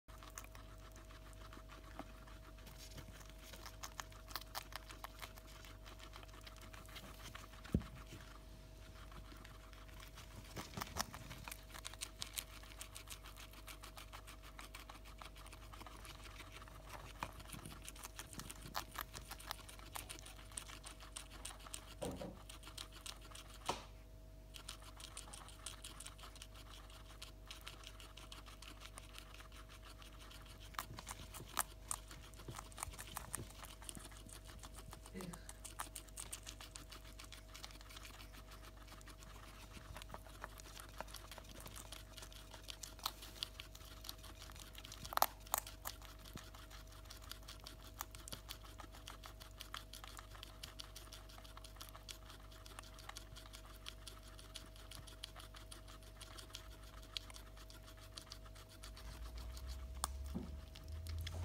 Theodore and Stanley show you how to enjoy breakfast very quickly
Theodore and Stanley the guinea pig boars love breakfast! Check out how fast Theo finish the french bean!